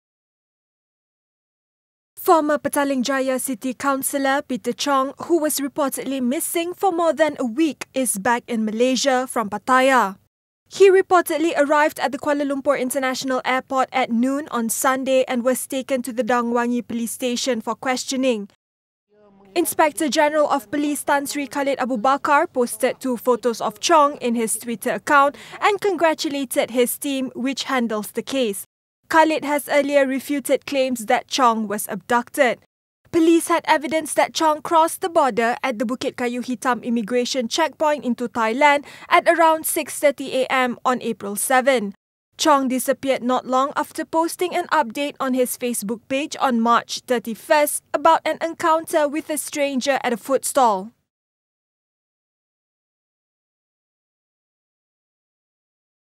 'Missing' activist Peter Chong arrives home after Thai trip
Former Petaling Jaya city councillor Peter Chong, who was reportedly missing for more than a week, has returned home.